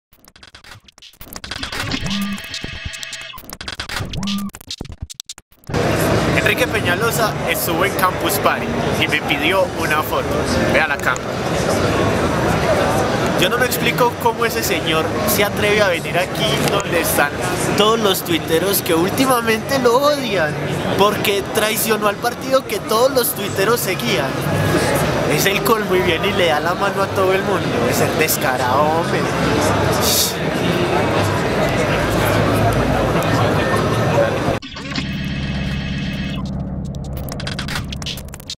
El mundo según JuanSe [Edición especial Peñalosa en CPCO4]
El candidato a la alcaldia de Bogotá por el Partido Verde Enrique Peñalosa estuvo en Campus Party y tuvo un pequeño encuentro que no dejó pasar de largo.